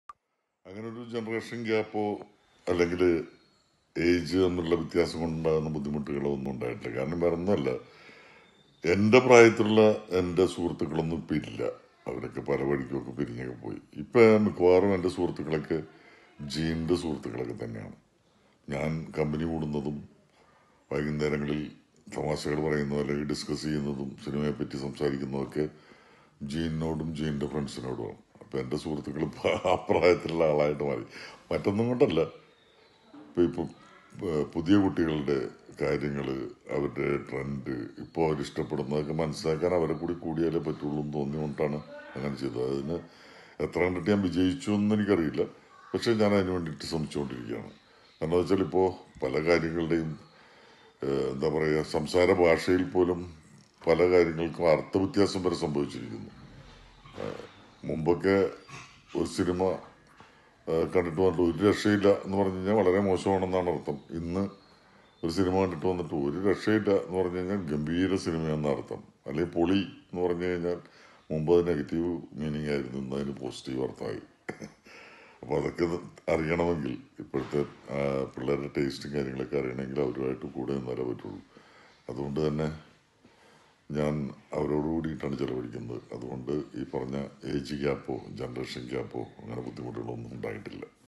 മകനൊപ്പം സിനിമ ചെയ്തപ്പോൾ ജനറേഷൻ ഗ്യാപ്പ് തോന്നിയിരുന്നോ ? ലാൽ മനസ്സ് തുറക്കുന്നു.
Tsunami || World Television Premire || Now Running || Lal || Interview || Asianet